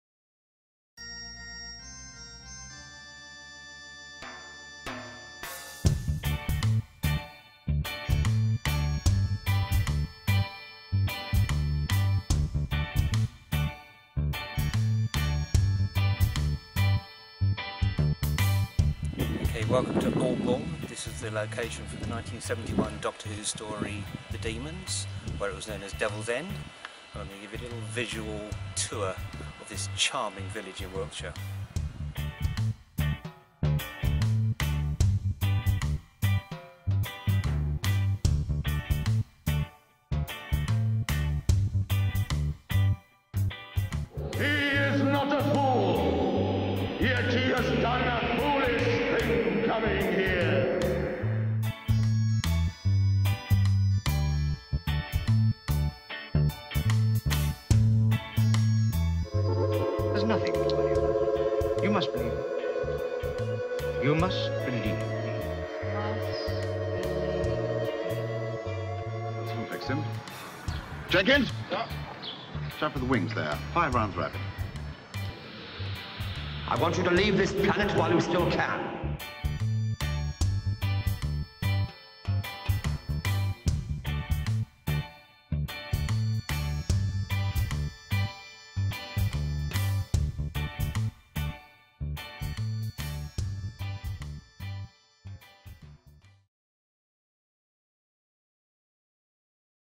Devils End Revisited - A trip to Aldbourne
On our way to the Avebury Monument in August 2015, Marq and Al stop by the charming Village of Aldbourne in Wiltshire. The Village was the central location for the 1971 Doctor Who story The Daemons starring Jon Pertwee and in this brief Video, we walk around some of the locations.

Filmed and Edited by Marq English